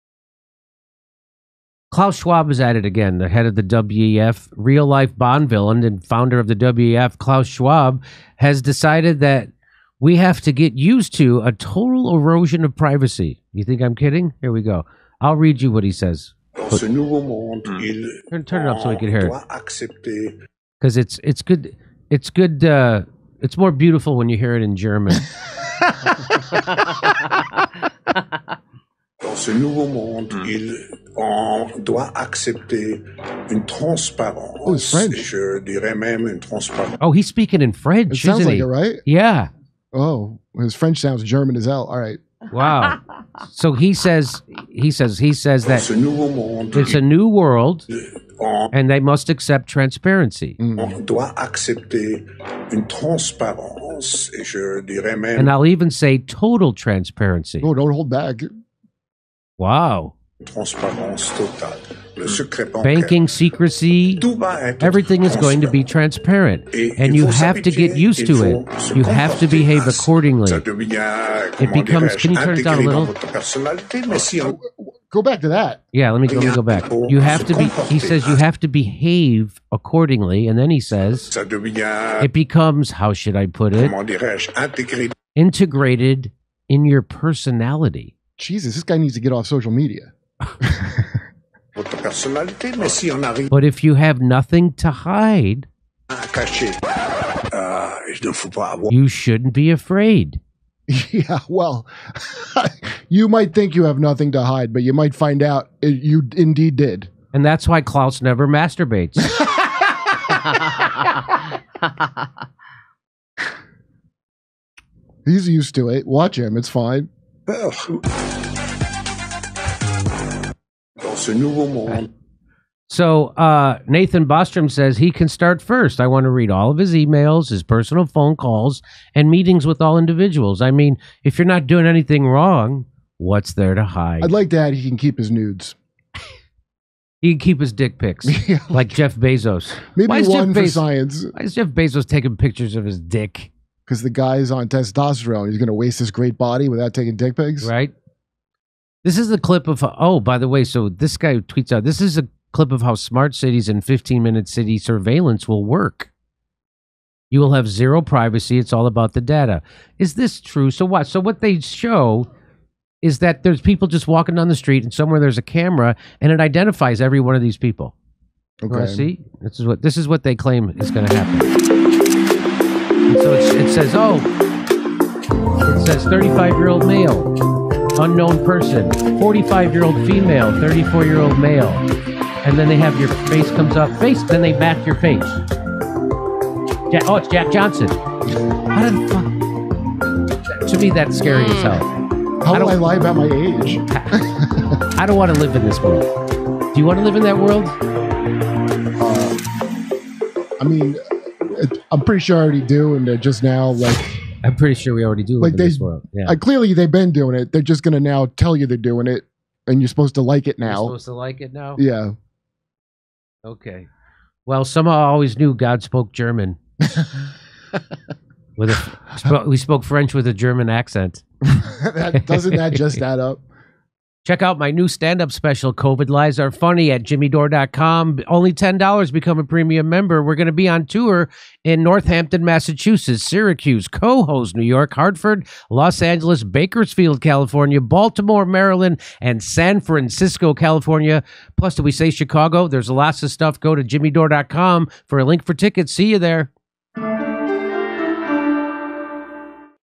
You’ll Have No Privacy And Like It! Says WEF Head Klaus Schwab
World Economic Forum head Klaus Schwab gets accused of sounding like a movie villain, and he does nothing to dispel the notion with his public statements. Most recently Schwab told an interviewer that privacy would soon be a thing of the past and that we should all embrace this bold future since, as long as we don’t have anything to hide we should have anything to worry about.

Jimmy and Americans’ Comedian Kurt Metzger discuss the brave new world people like Klaus Schwab say we should accept even if they won’t be subject to its dictates themselves.